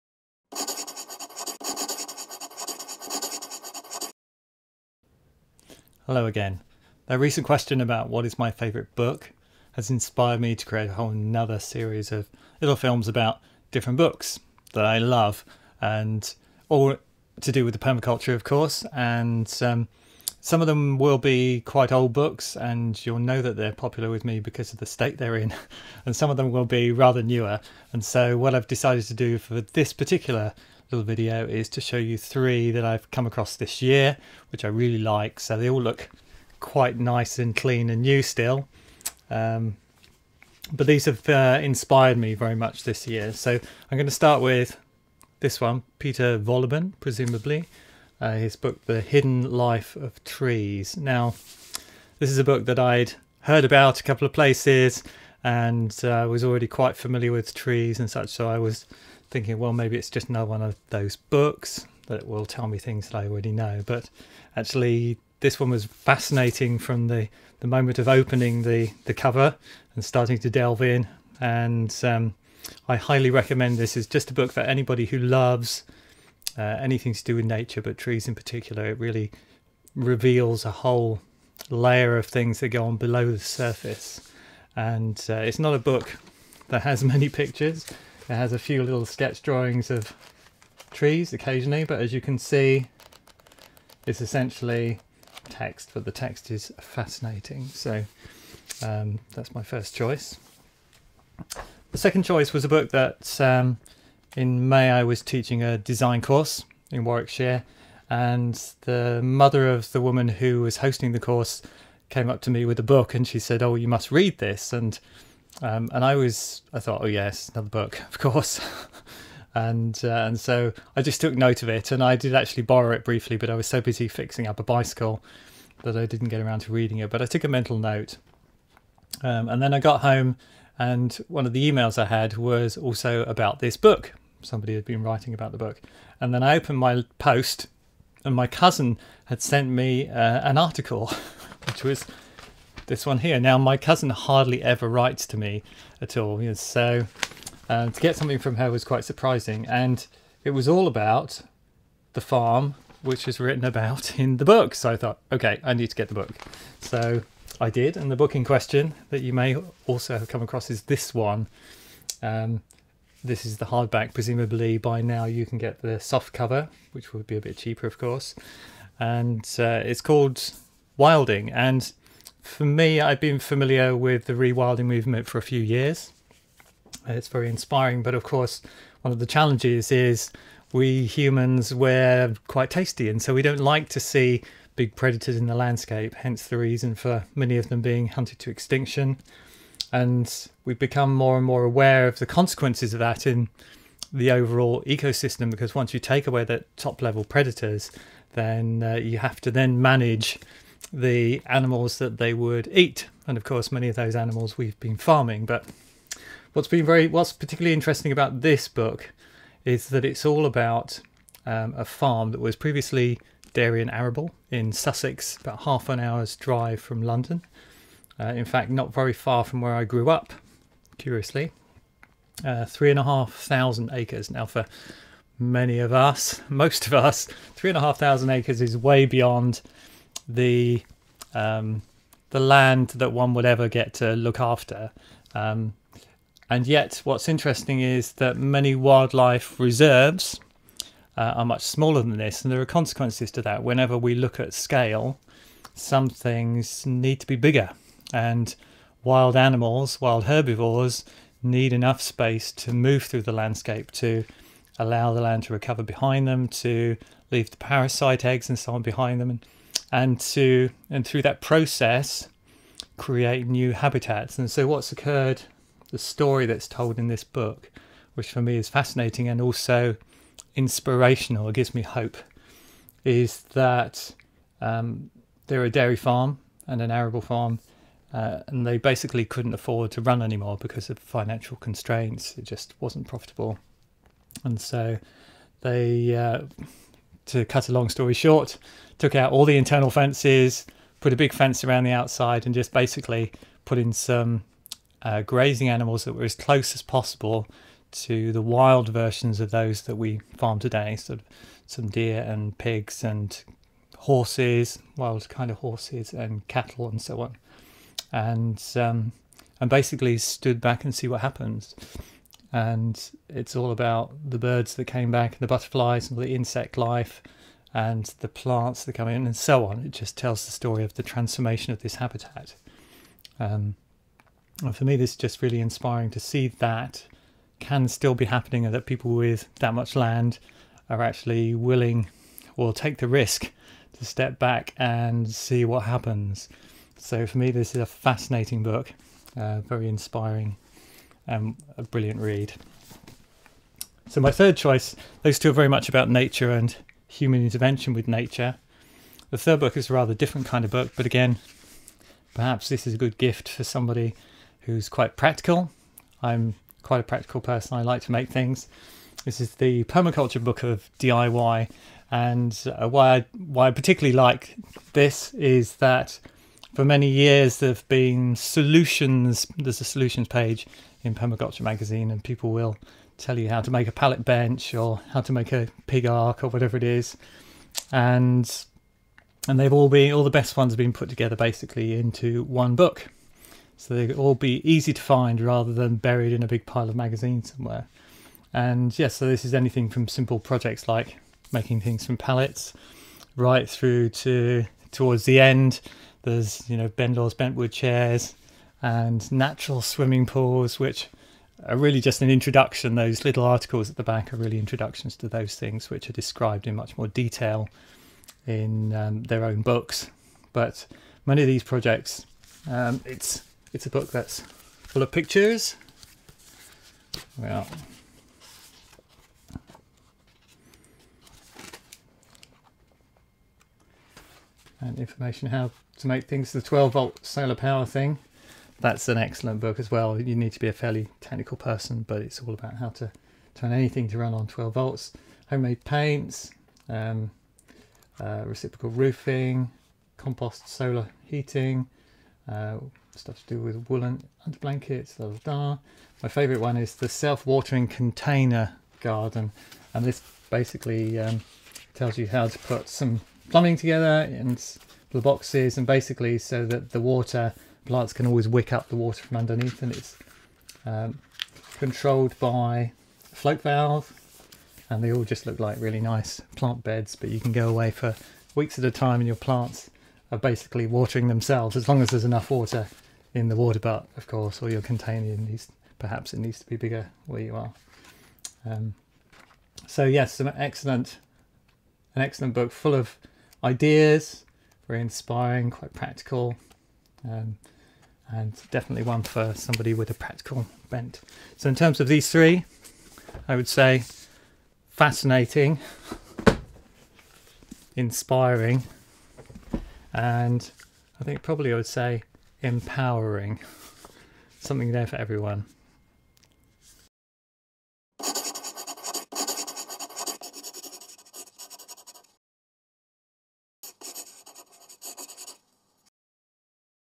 Three books for gifting
Aranya shares some of his favourite books. This time three books that would make great gifts - perhaps even for yourself!